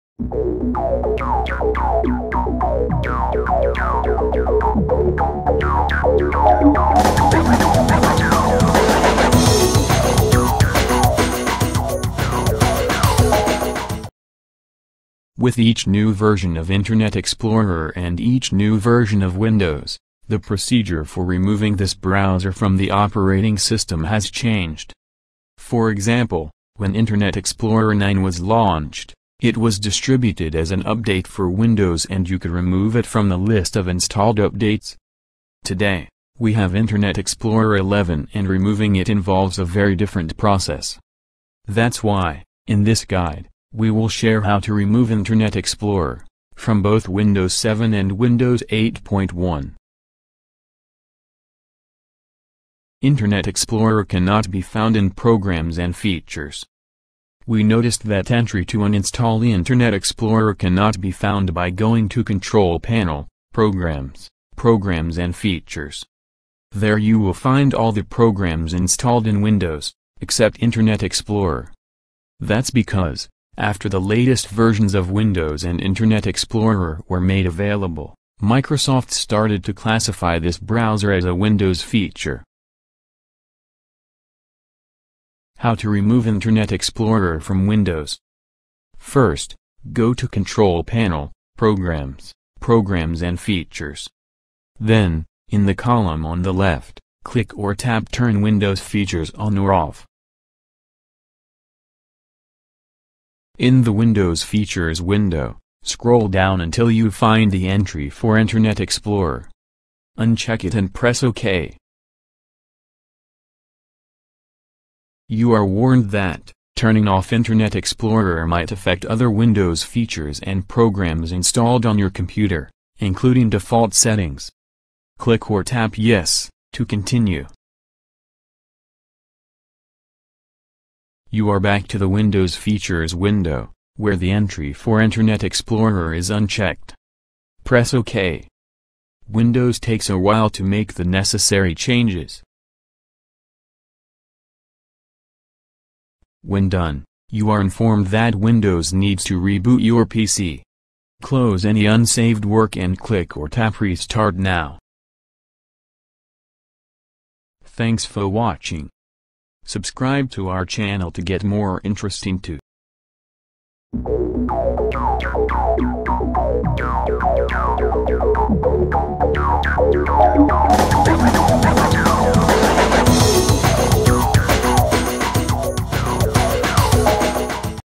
How To Remove Internet Explorer From Windows 7 And Windows 8.1
Uninstall, Disable, or Delete Internet Explorer from Windows 7 And Windows 8.1

A lot of Windows users out there dislike Internet Explorer enough that just using a different browser is not enough, they want it gone.  Although there is not a way to completely uninstall it, let’s take a look at how to disable IE 8 in Windows 7 so you won’t have to deal with it anymore.